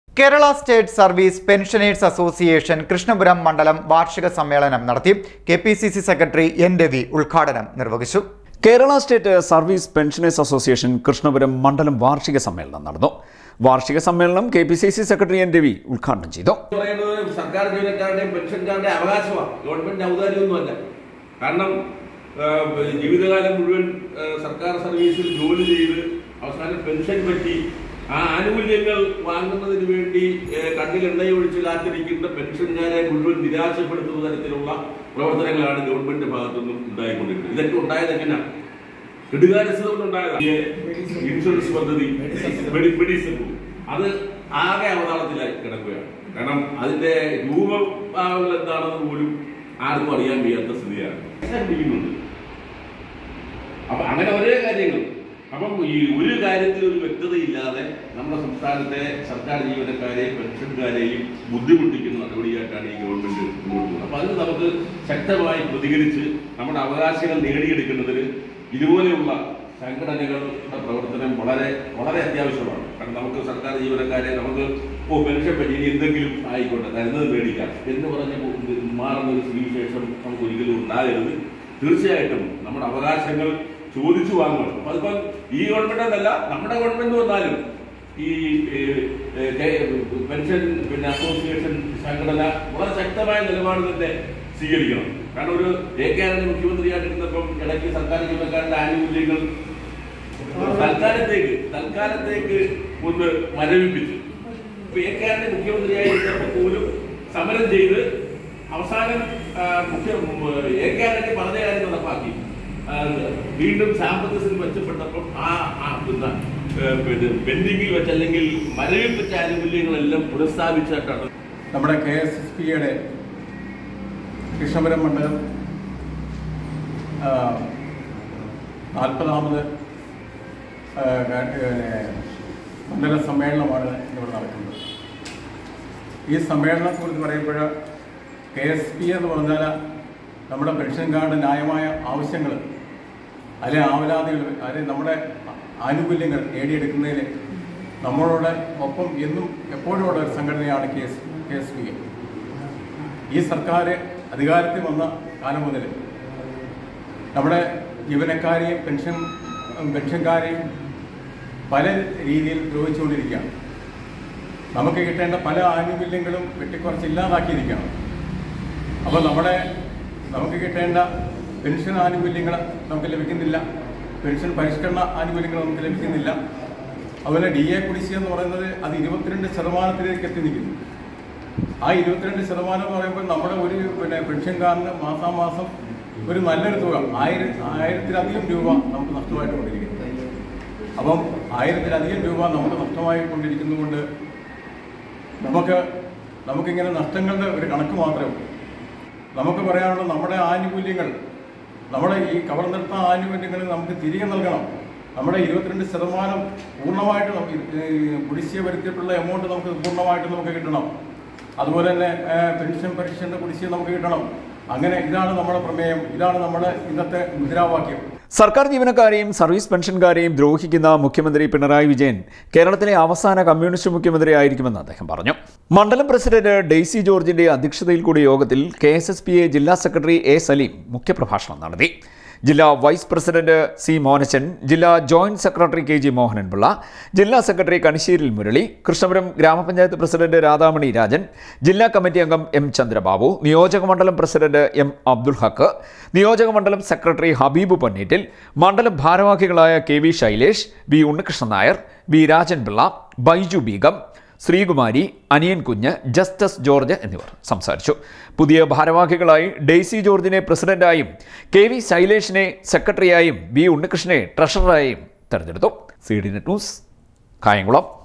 കേരള സ്റ്റേറ്റ് സര്‍വ്വീസ് പെന്‍ഷനേഴ്‌സ് അസോസിയേഷന്‍ കൃഷ്ണപുരം
കേരള സ്റ്റേറ്റ് സര്‍വ്വീസ് പെന്‍ഷനേഴ്‌സ് അസോസിയേഷന്‍ കൃഷ്ണപുരം മണ്ഡലം വാര്‍ഷിക സമ്മേളനം നടത്തി. കെ.പി.സി.സി. സെക്രട്ടറി എന്‍. രവി ഉദ്ഘാടനം നിര്‍വഹിച്ചു.